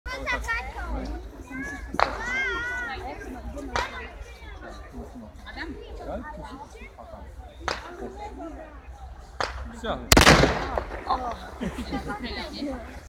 Sofiin výstrel
Po programe Rekonštrukcia bojov o Bardejov sa ponúkla aj táto možnosť